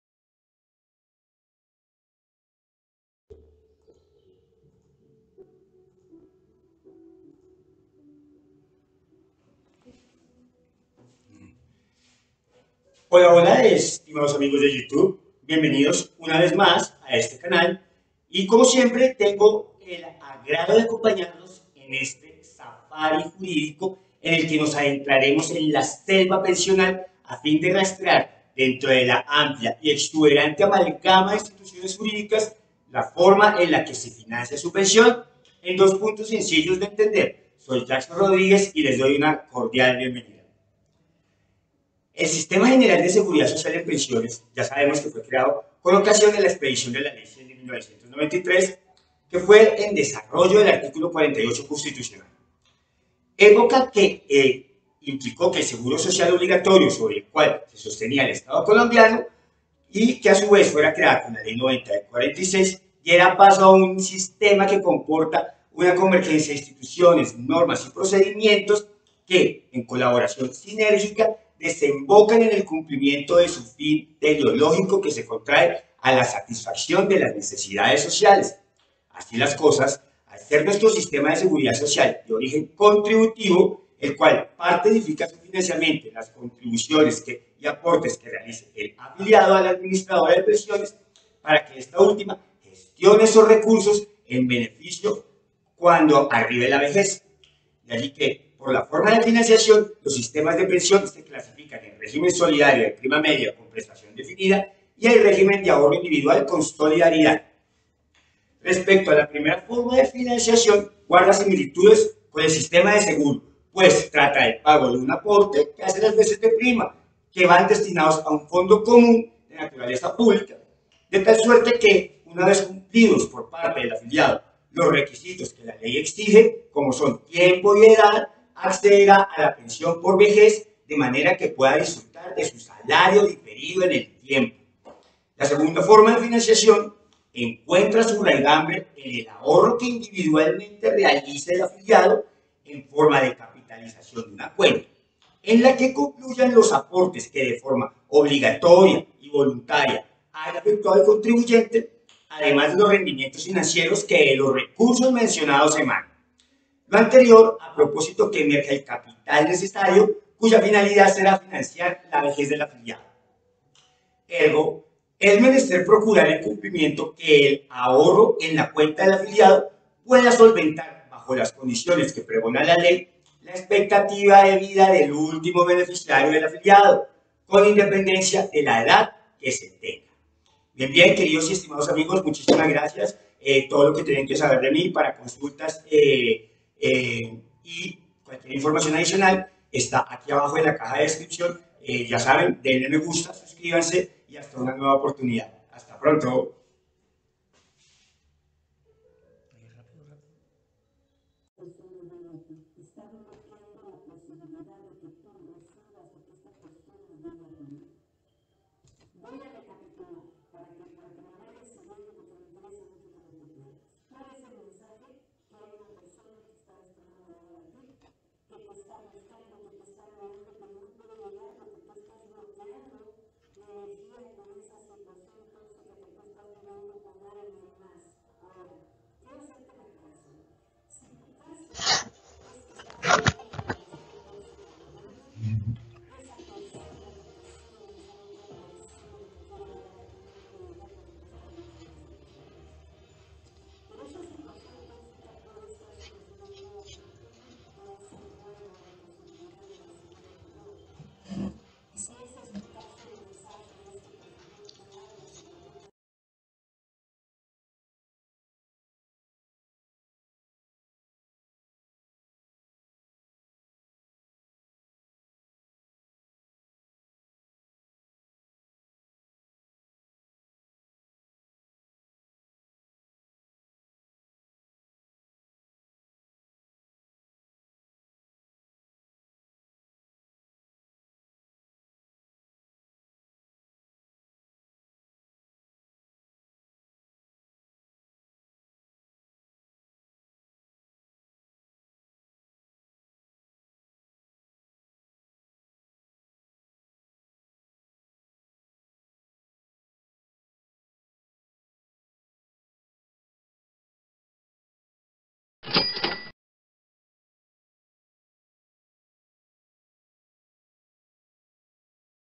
PONGA MUCHA ATENCIÓN COMO SE FINANCIA SU PENSIÓN
la forma de financiación que opera a la égida del Sistema de Seguridad Social, los sistemas de pensión se clasifica en el Régimen Solidario de Prima Media con Prestación Definida y el Régimen de Ahorro Individual con Solidaridad.